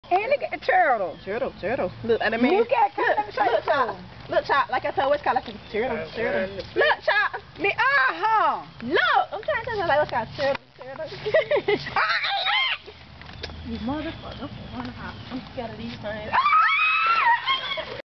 Thom the Turtle
A turtle we saw in the yard when we came home. We have no idea how it got there because there are no rivers, lakes, or any kind of body of water near us. Then Ann picked it up, and Kim & Brit sreamed and ran away. lol